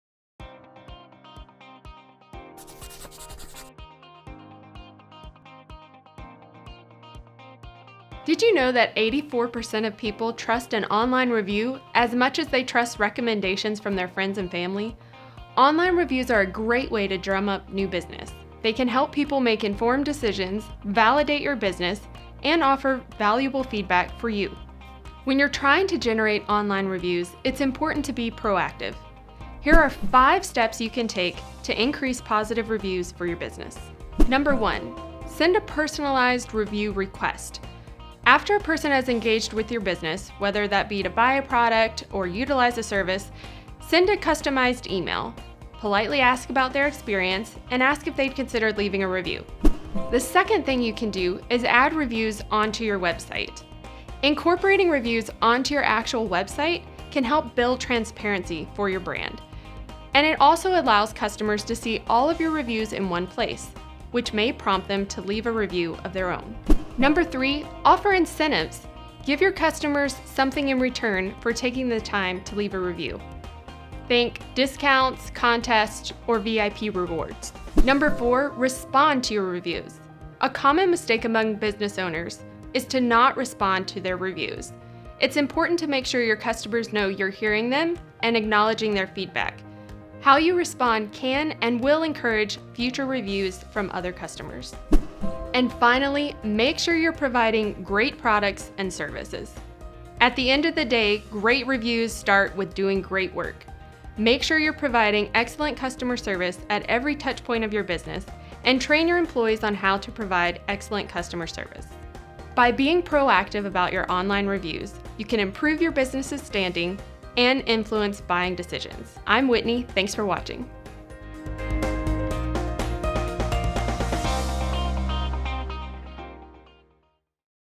The Rundown: How To Generate Customer Reviews
When you’re trying to generate more customer reviews, it is important to be proactive! In this video, we discuss 5 steps your business can take to increase positive reviews from your customers.